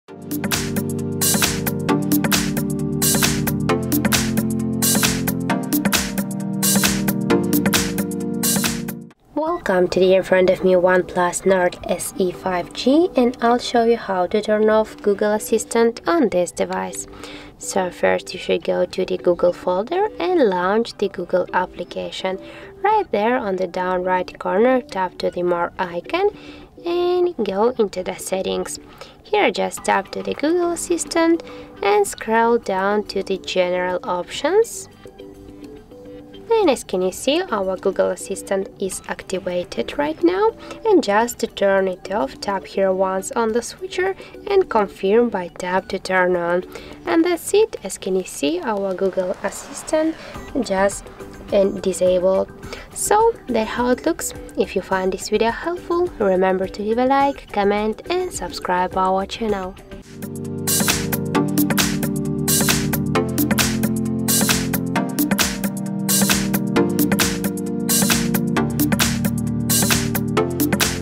How to Turn Off Google Assistant in OnePlus Nord CE 5G – Disable Voice Assistant
Learn more about OnePlus Nord CE 5G
Hello! Have you ever wondered how you can disable Google Assistant on your OnePlus Nord CE 5G? If you haven't already seen it, make sure you watch our new video. We'll show you in this video how you can disable Google Assistant in a few easy simple steps. Google Assistant is very useless and annoying to you, we get it, here to help! You can turn off Google Assistant on your OnePlus Nord CE 5G, all you need to do is watch this short tutorial to the end, follow the instructions provided by our expert, and try this yourself.